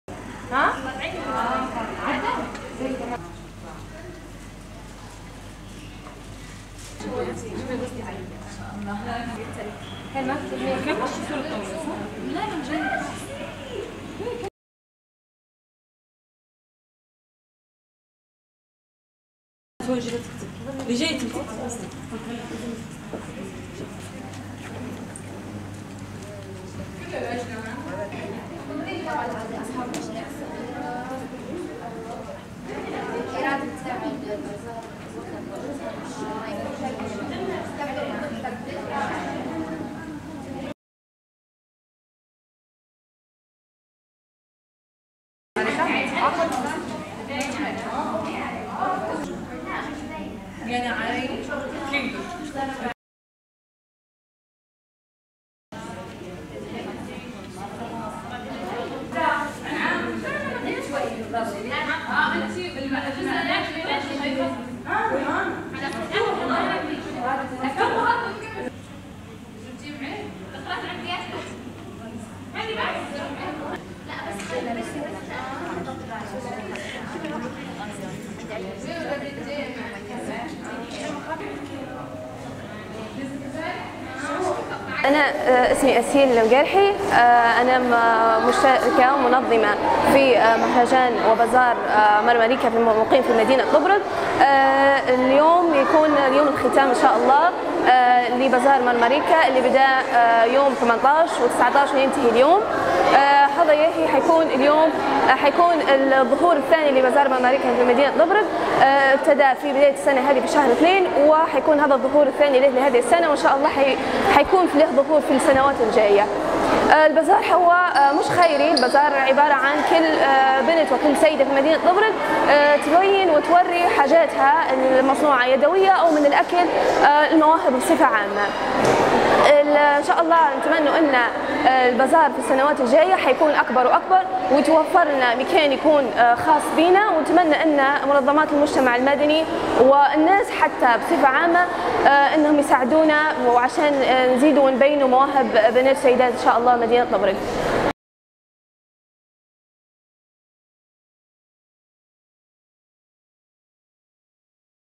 اختتام بازار (مارماريكا) الخيري بطبرق LCNA
وكالة الغيمة الليبية للأخبار – طبرق.

اختتمت في مدينة طبرق فعاليات (بازار مارماريكا) الخيري بفندق دار السلام، الذي يهدف لإظهار الأفكار والمواهب والأعمال اليدوية والمشاريع الصغرى بحضور عدد كبير من نساء طبرق.

وأكدت إحدى المنظمات للبازار أن اللجنة المنظمة له سوف تخصص الجزء الأكبر من عائد المبيعات لصالح تنمية المشروعات الناشئة والأعمال الحديثة بمدينة طبرق؛ مشيرة إلى أن البازار يساهم في إظهار المواهب في الجوانب الفنية والتراثية المختلفة.